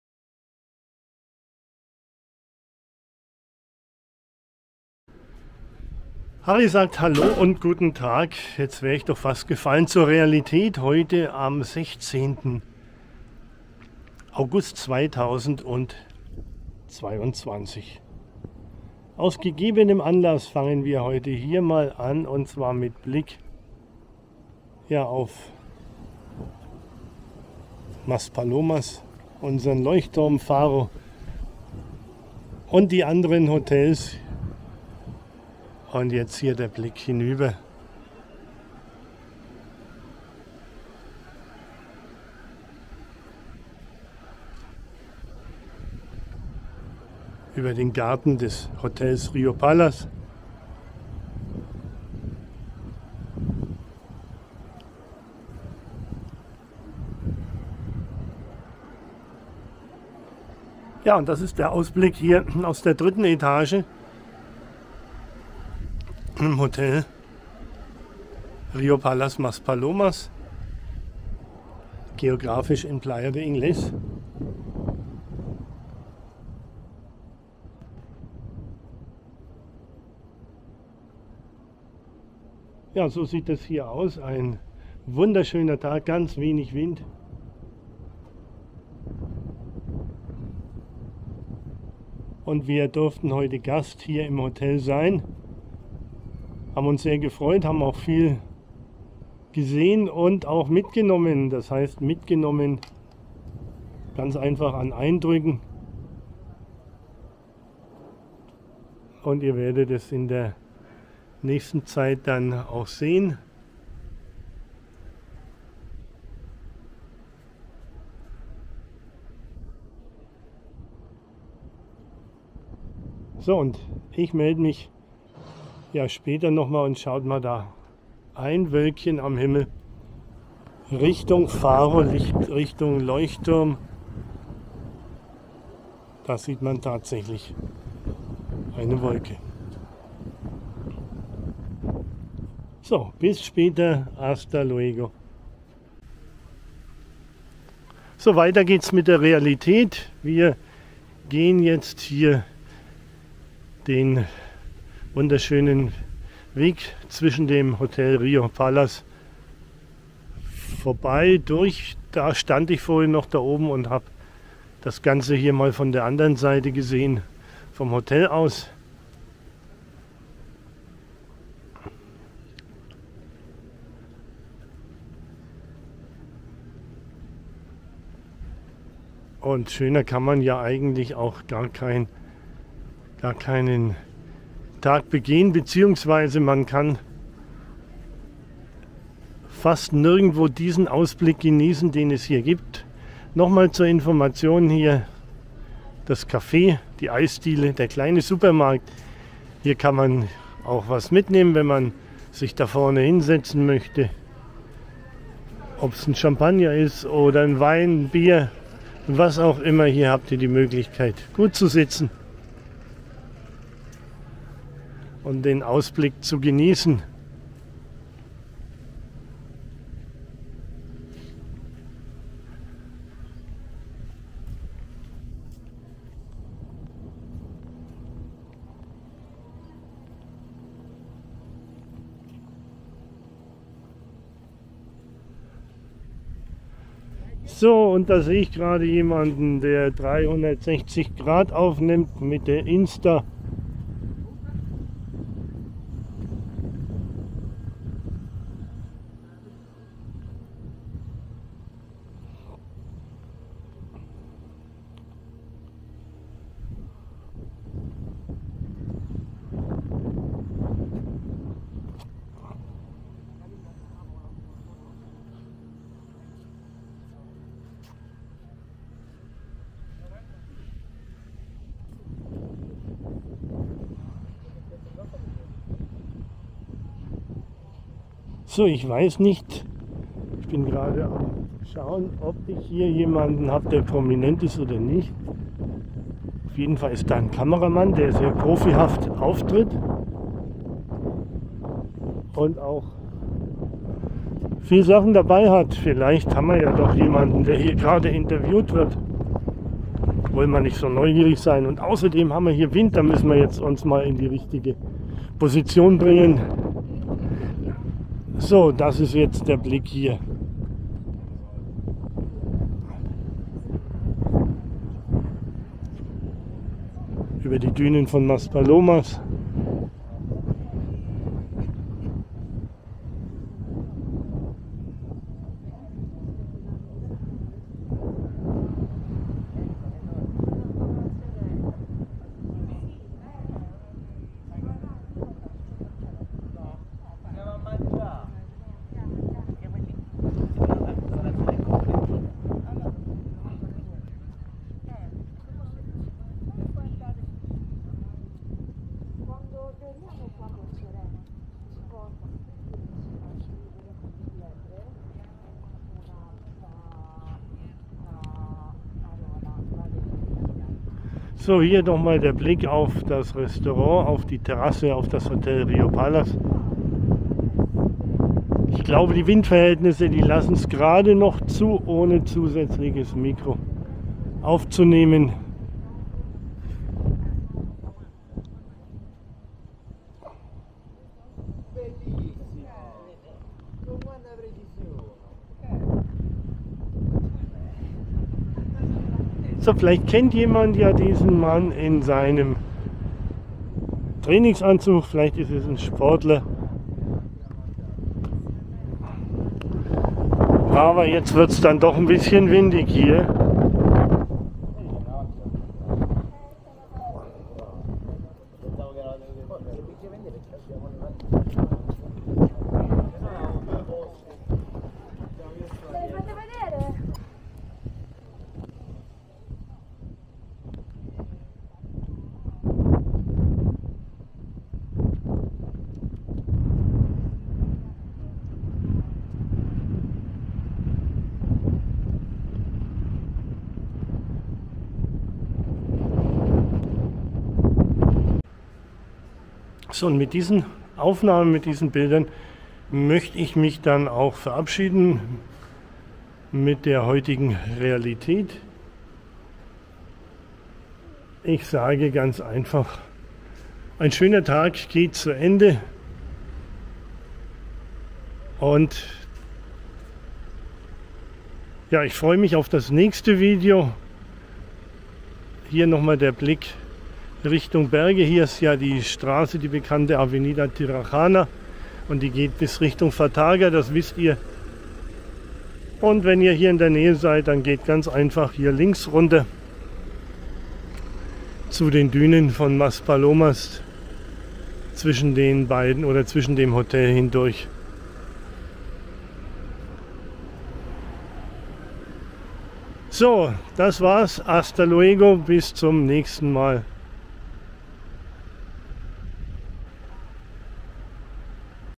Realität Gran Canaria Maspalomas 16.08.22 Ab heute ist Fataga für 10 Tage nur sehr mühselig zu ....
Ab heute ist Fataga für 10 Tage nur sehr mühselig über die GC-65 zu erreichen.
Danke schön.

Gracias

Spanisch lernen mit Conny und Land und Leute
Connys Kanal: